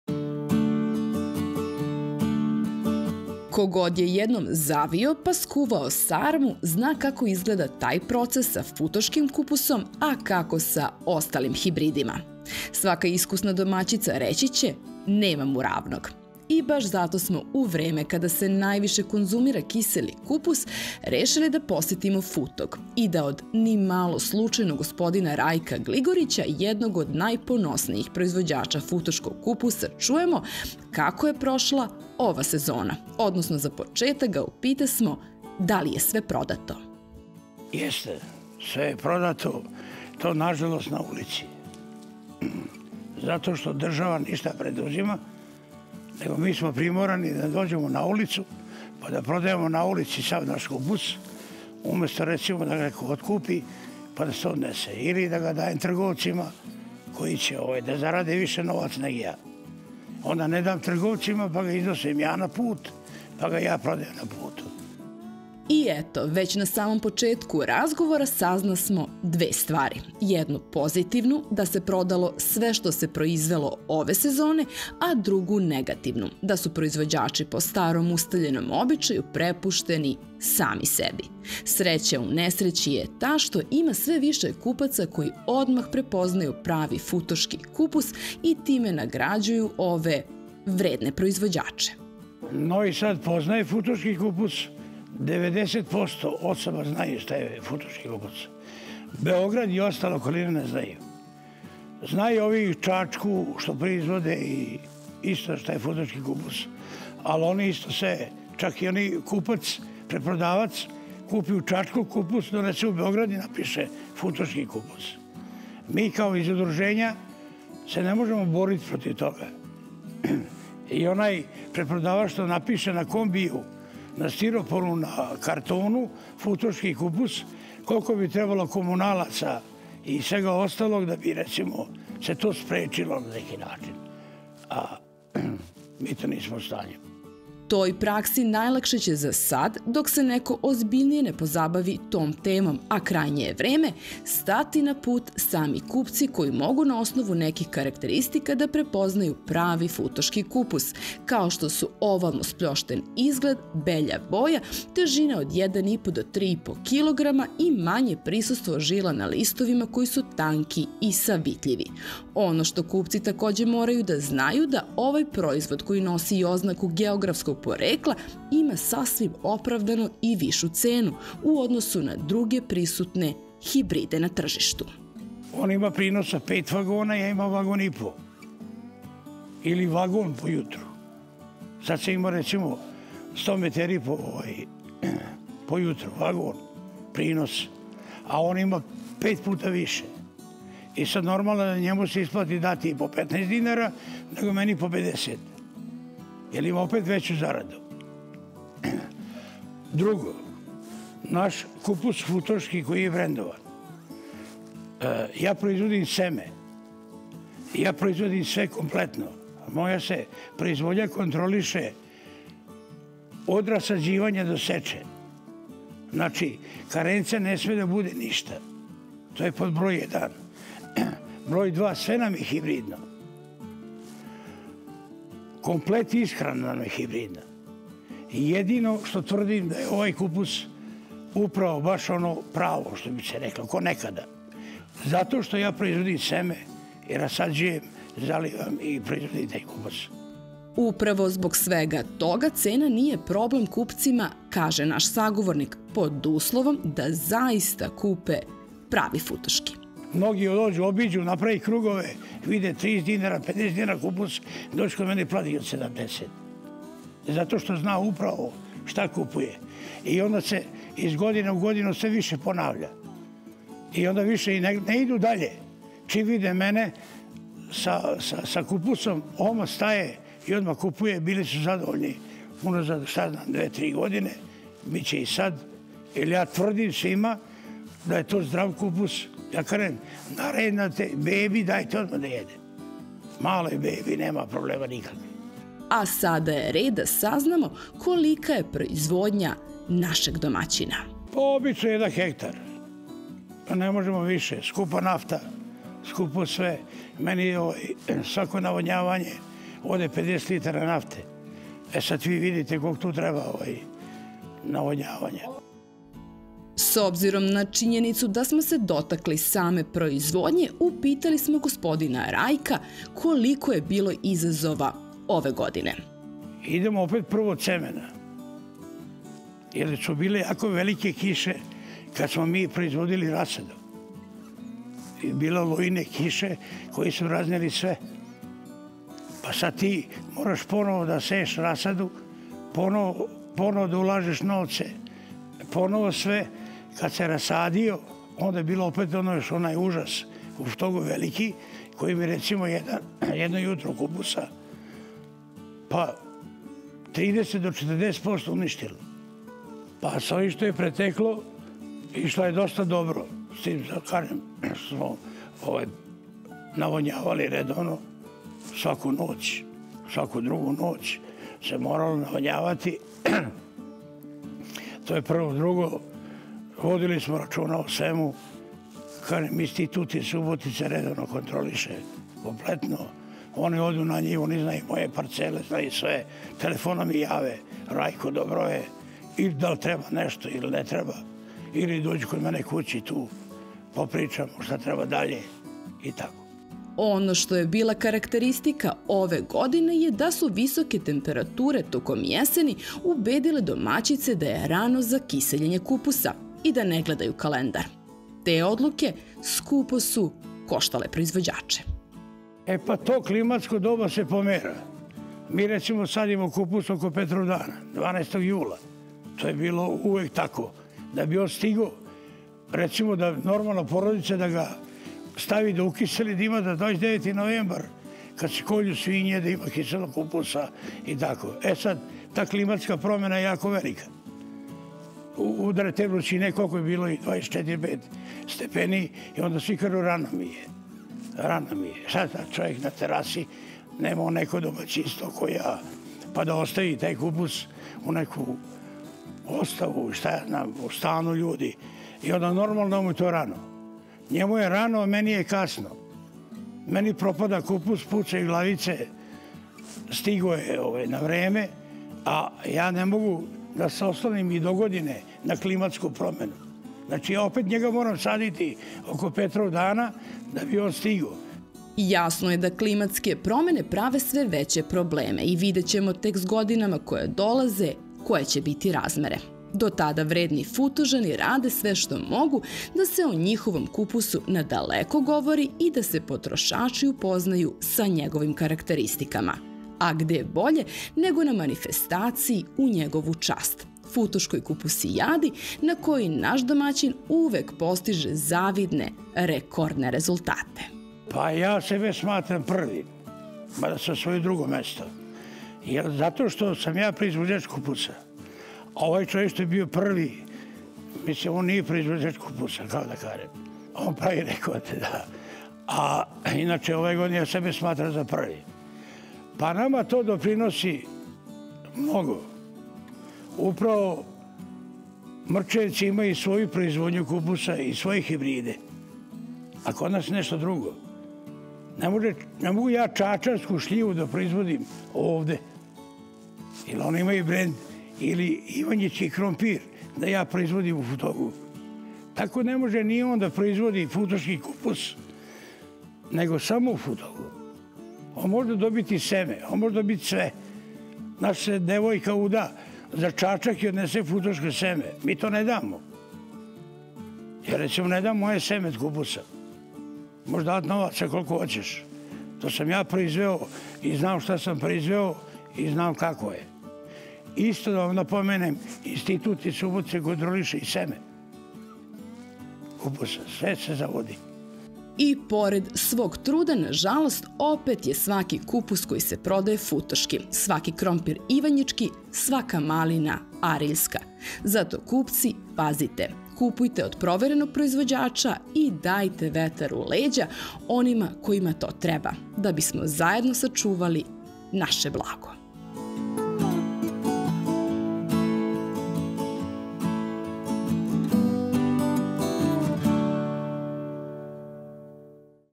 Nije svaki kupus FUTOŠKI! Važno je znati koji je pravi!
Po čemu će ovu sezonu pamtiti proizvođači futoškog kupusa?
Sagovornik: Rajko Gligorić – Futog
Facebook: Farma emisija o poljoprivredi